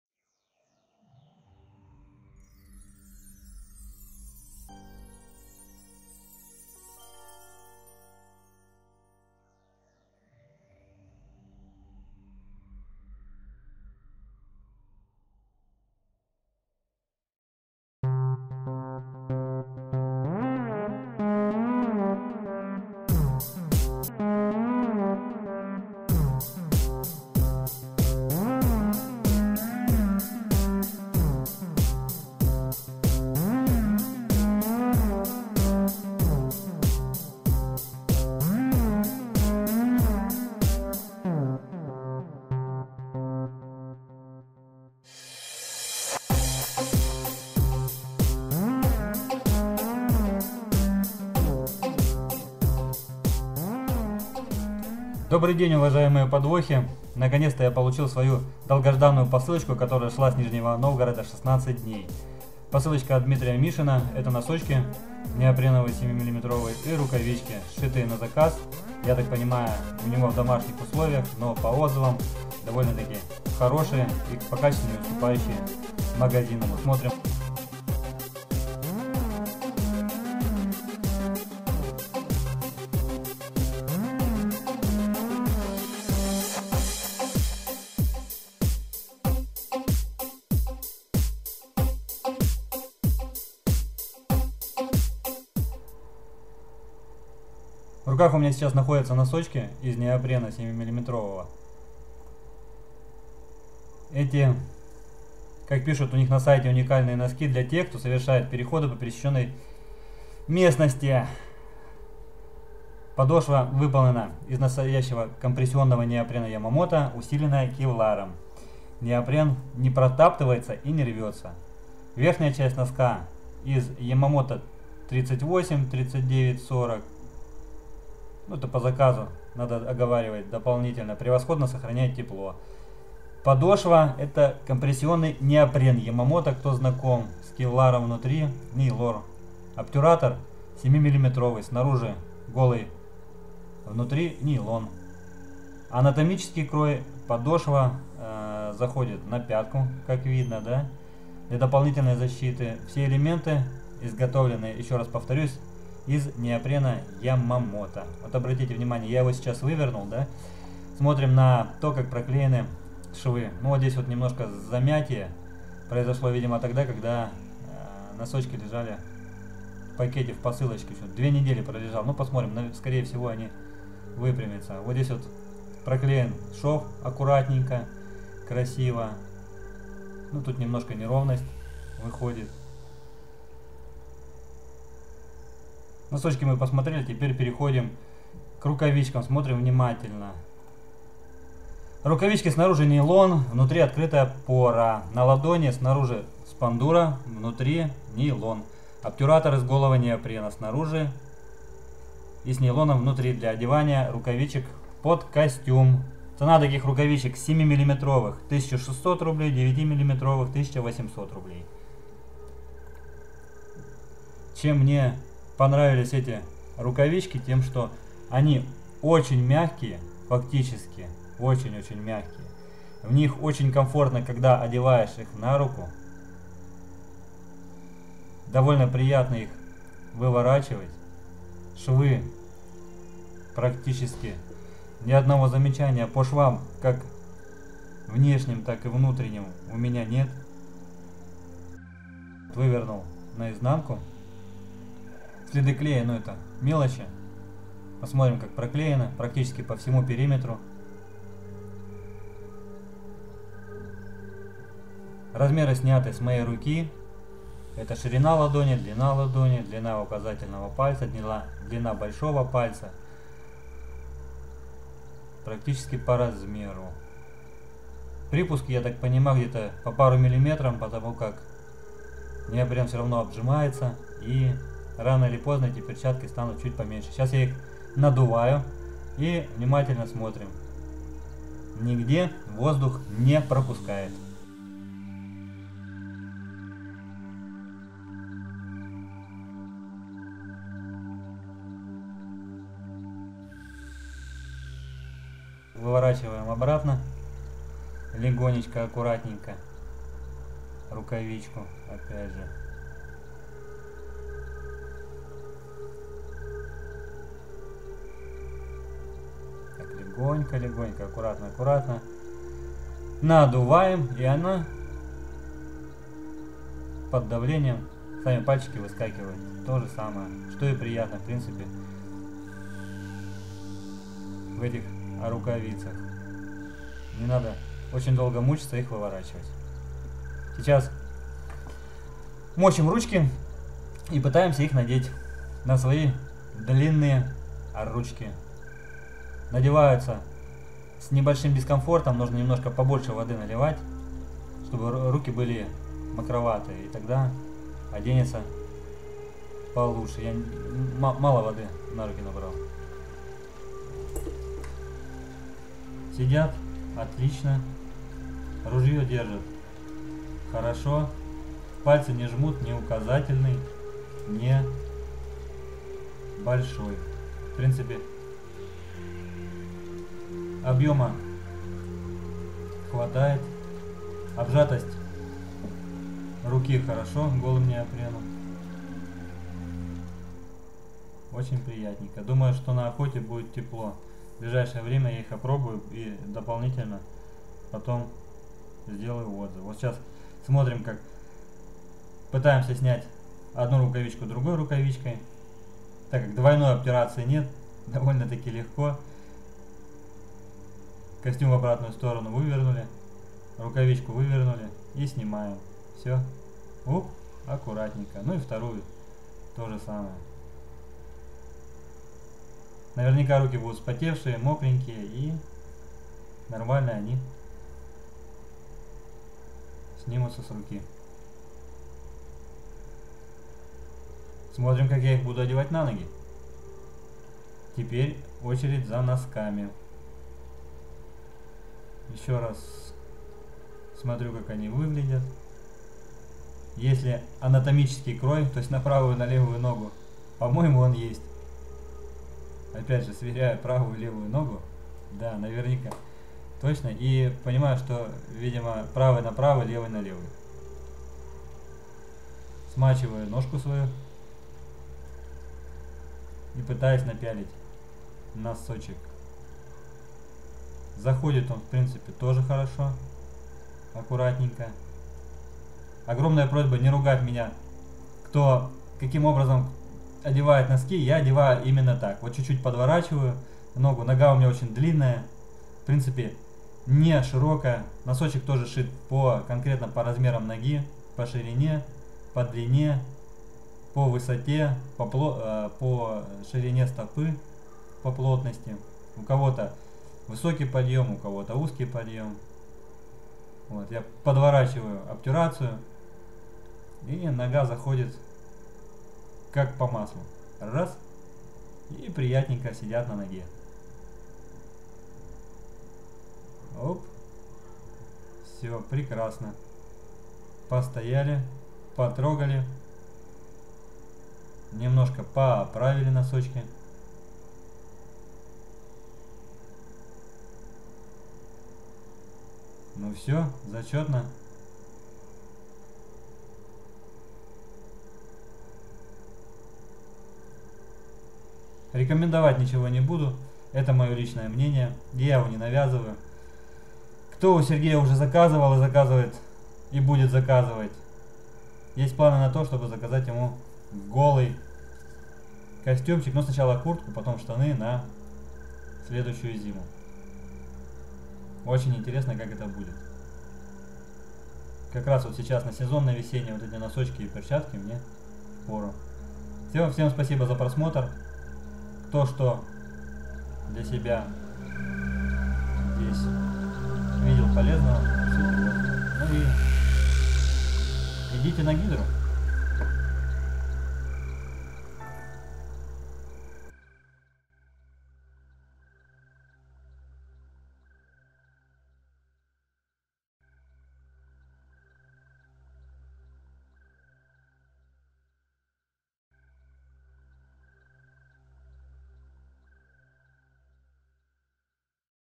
Подводная охота. Снаряжение для подводной охоты от Гидры. Неопреновые носки и перчатки на заказ.
Обзор на неопреновые носки и перчатки от "Гидры".
Надеюсь что Сергей меня не "побьет" за обзор на его продукцию от первого лица.
группа в контакте!
ГИДРА
производство и продажа неопренового снаряжения для подводной охоты